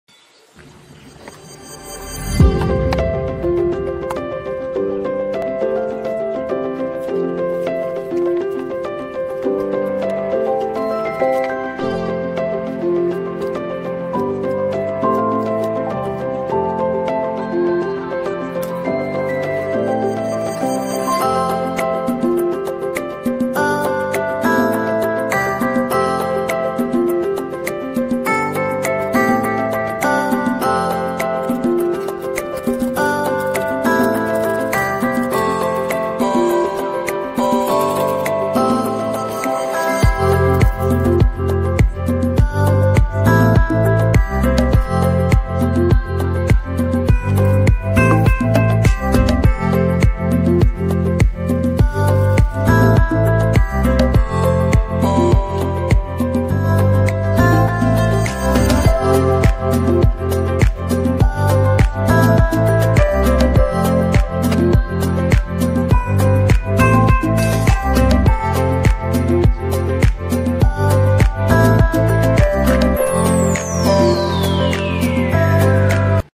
Excavating Adventure gemstone digging
This is the perfect gift for little ones who loves to dig! You will discover new rocks, fossils, minerals, gemstones, seashells as you enjoy unique and exciting adventures created only for the most curious and adventurous minds. Every specimen comes with a collectible identification card. Collect ten new specimens every month and watch your collections grow!

Thank you @excavatingadventures for sending this to us! 🌼

Musician: iksonmusic